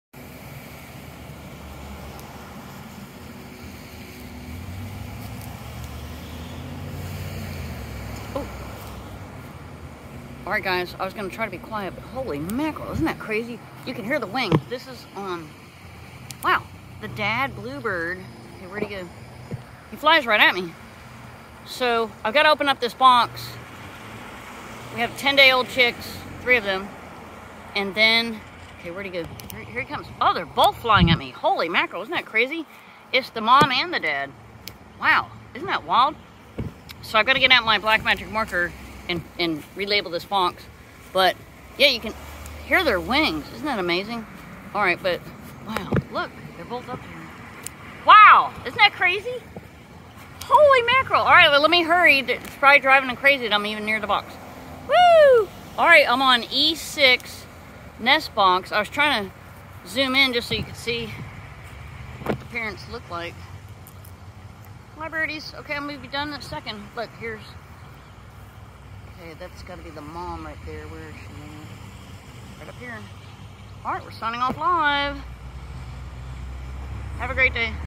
Dad Bluebird & Mom Dive Bomb Me & Hear Wings as They Pass Over E6 Nestbox Sea Pines Bluebird Project
Dad Bluebird & Mom Dive Bomb Me & Hear Wings as They Pass Over E6 Nestbox Sea Pines Bluebird Project! Hilton Head Island, SC